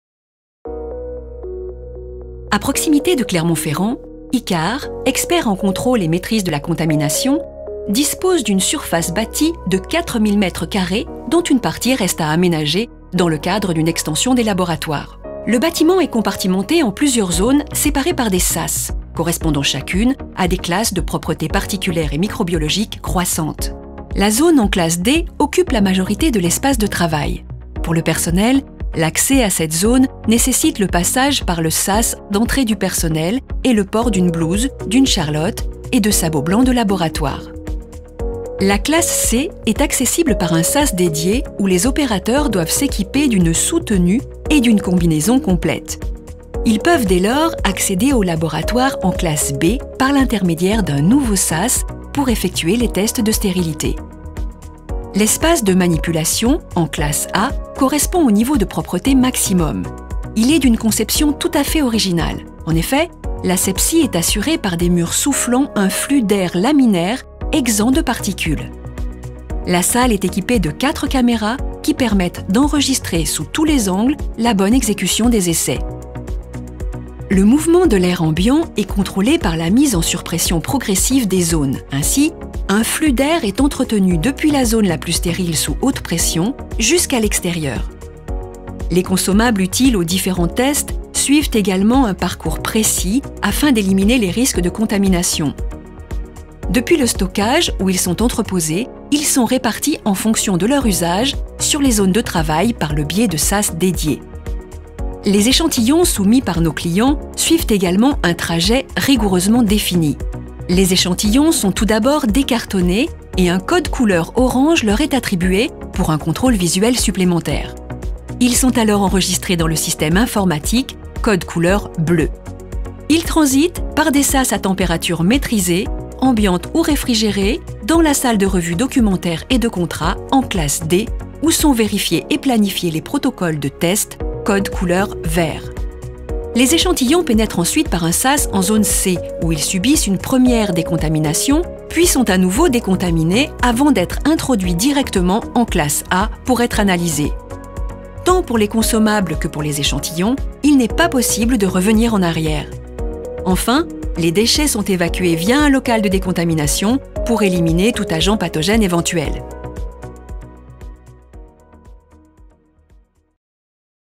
LABORATOIRES ICARE FRA
Le Groupe Icare présente une visite des locaux de son siège social à Saint-Beauzire. Des laboratoires de classe D à A, avec une surface totale de 4000 m2.

Découvrez l’évolution des experts et des produits de santé au sein des laboratoires Icare.

The Groupe Icare shows you around the premises of its head office in Saint-Beauzire. Class D to A laboratories, with a total surface area of 4000 m2.
Find out more about the evolution of healthcare products experts and healthcare products within laboratories.